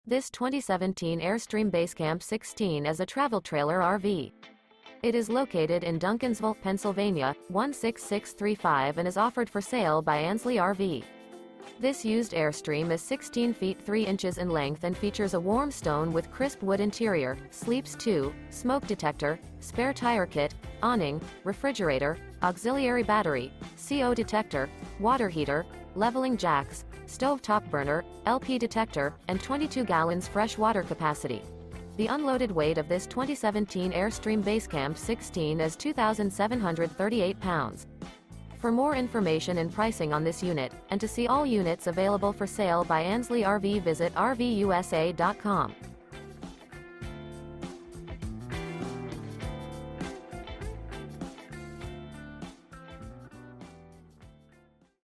Eye-catching 2017 Airstream Basecamp Travel Trailer RV For Sale in Duncansville, PA | RVUSA.com
This 2017 Airstream Basecamp 16 is a Travel Trailer RV. It is located in Duncansville, PA 16635 and is offered for sale by Ansley RV. This Used Airstream Is 16' 3" in length and features a Warm Stone w/ Crisp Wood interior, sleeps 2, Smoke Detector, CO Detector, Awning, Leveling Jacks, Refrigerator, Stove Top Burner, Auxiliary Battery, Spare Tire Kit, Water Heater, External Shower, and 22 gal fresh water capacity.The unloaded weight of this 2017 Airstream Basecamp 16 is 2738 lbs.